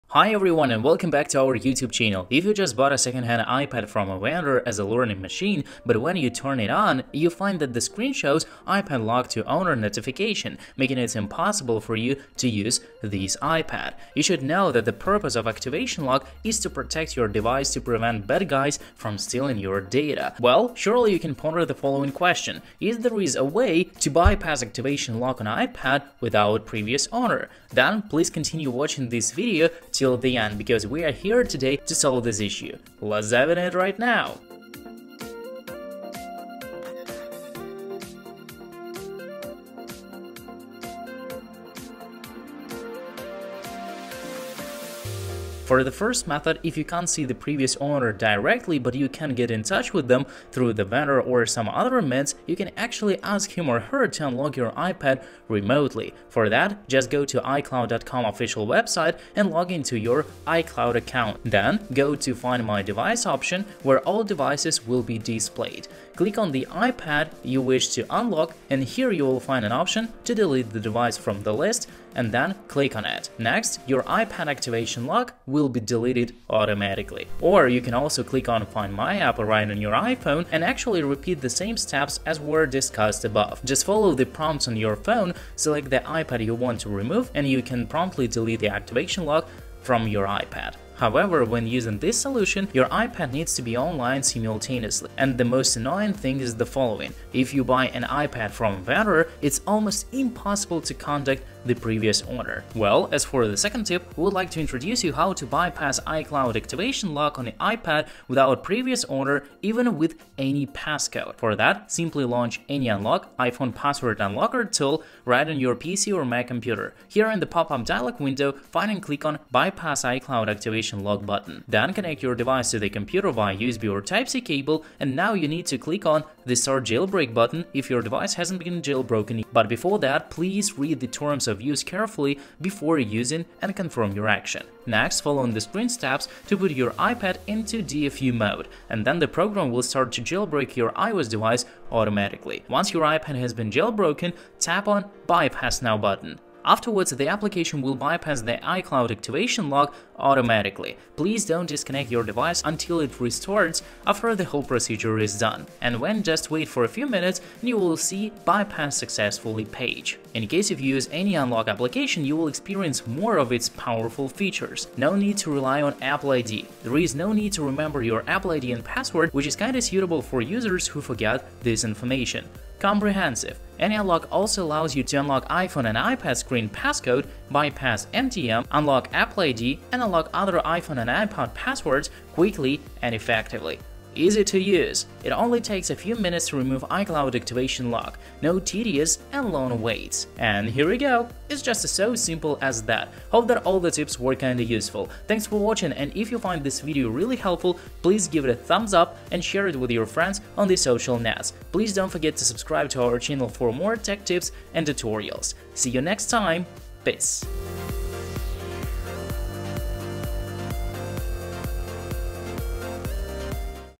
[1000% Success!] How to Bypass Activation Lock on iPad without Previous Owner | 2025 New Methods ✅
If your iPad is locked with Activation Lock and you cannot contact the Previous Owner. So is there a way to bypass Activation Lock on iPad without Previous Owner? Then please continue watching today's video.

If you just bought a second-hand iPad from a vendor as a learning machine, but when you turn it on, you find that the screen shows iPad locked to owner, making it impossible for you to use this iPad. You should know that the purpose of activation lock is to protect your device to prevent bad guys from stealing your data. Don’t worry, AnyUnlock will help you.
00:00 Intro
00:47 Tip 1. Bypass Activation Lock by iCloud
01:44 Tip 2. Bypass Activation Lock by AnyUnlock
02:25 Why people choose AnyUnlock